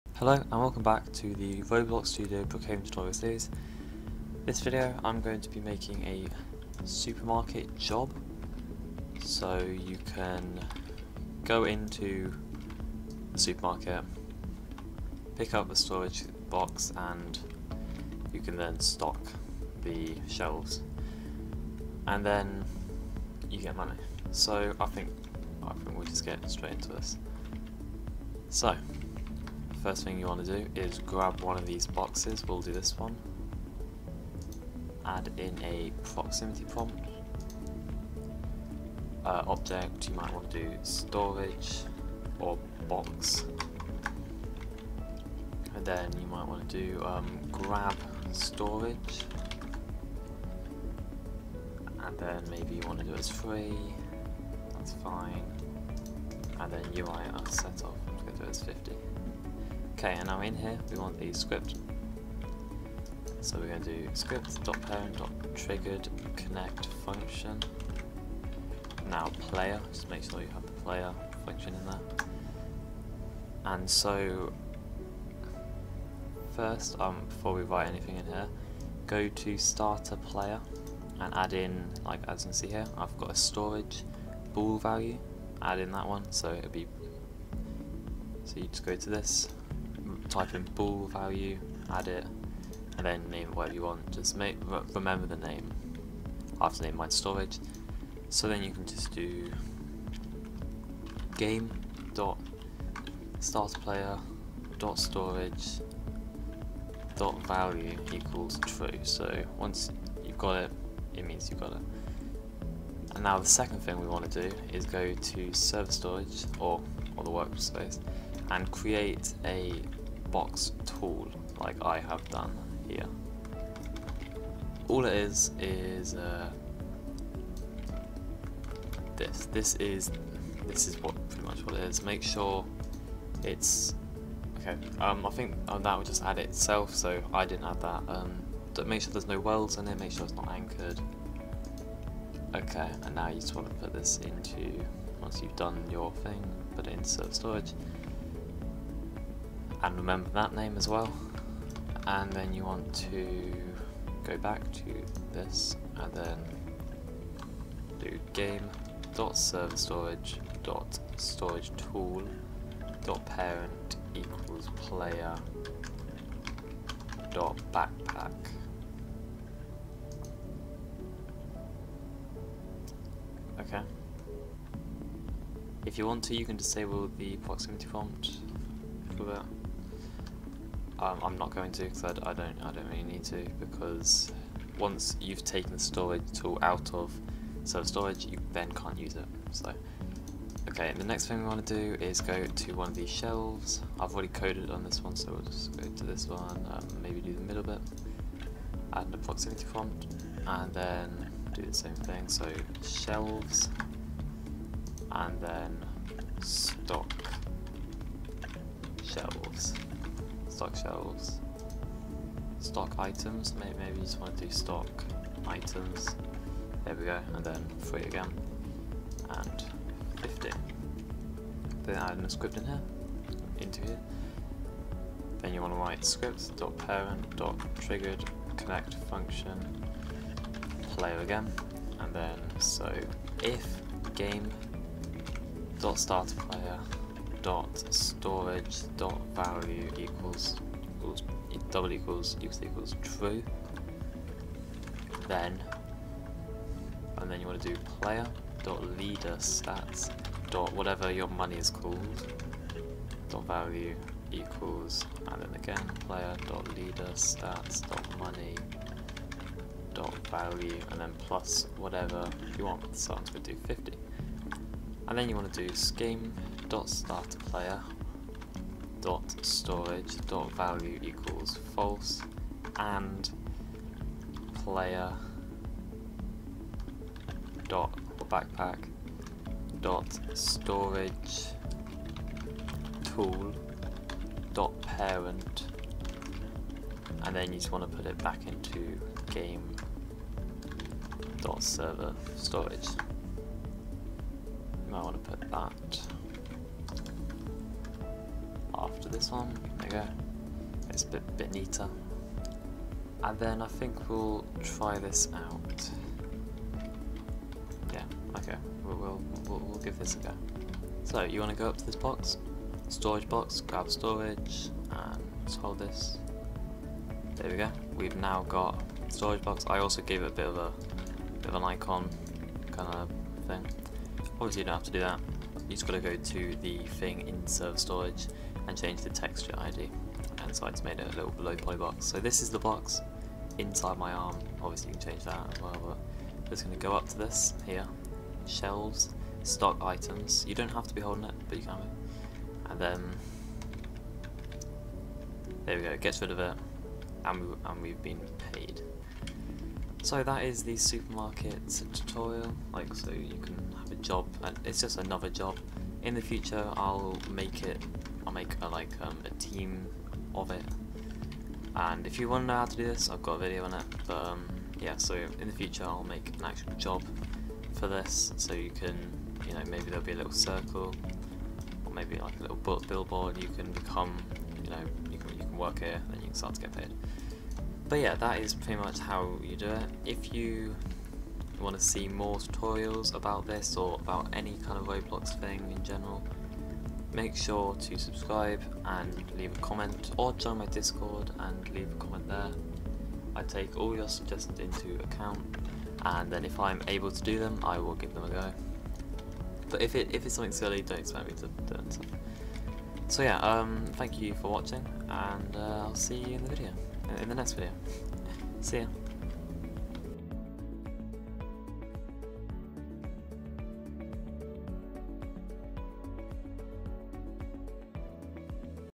How To Make A Game Like Brookhaven RP | How To Make A Supermarket Stocking Job | Part 20
How To Make A Game Like Brookhaven | Part 20

This is a Roblox Studio tutorial on how to make a Town and City game on Roblox. I hope you enjoy and learn how to make your very own Roblox game. This series has a data saving tutorial and a video on how to monetize your game by adding gamepasses and being able to earn robux. I hope you enjoy the videos.